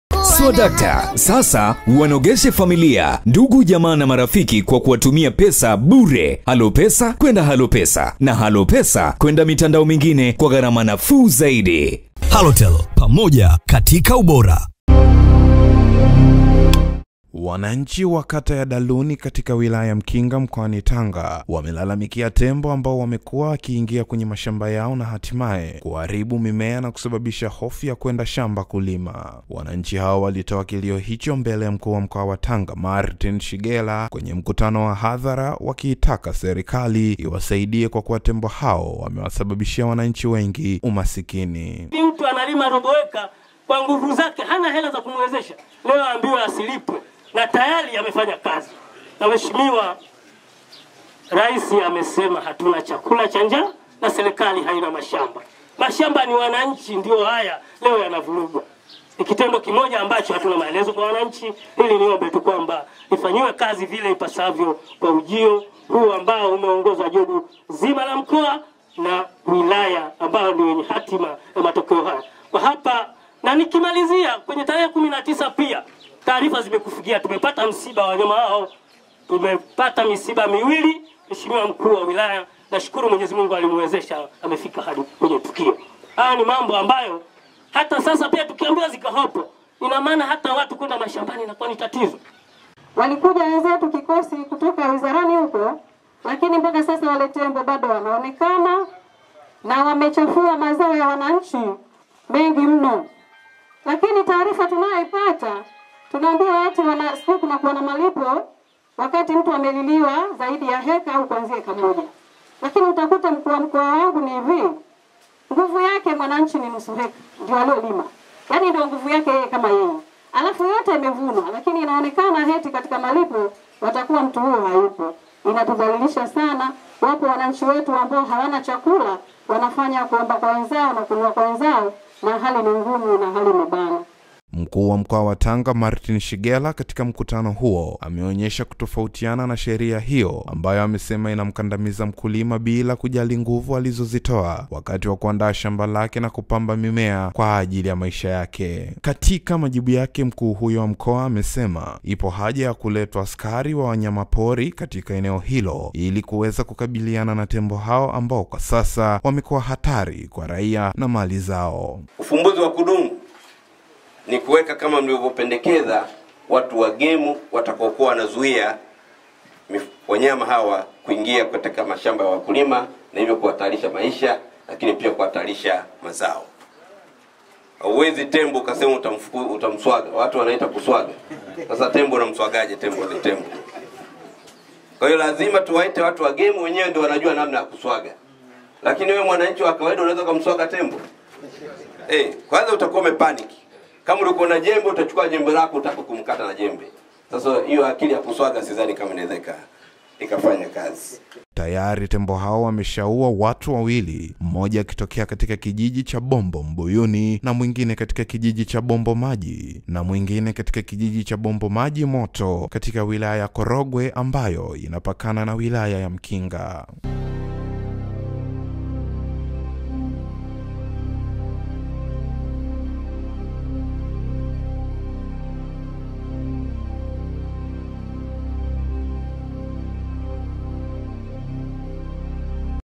TEMBO WALIZA WANANCHI MKOANI TANGA
Wananchi wa kata ya Daluni katika wilaya ya Mkinga Mkoani Tanga wamelalamikia tembo ambao wamekuwa wakiingia kwenye mashamba yao na hatimaye kuharibu mimea na kusababisha hofu ya kwenda shamba kulima.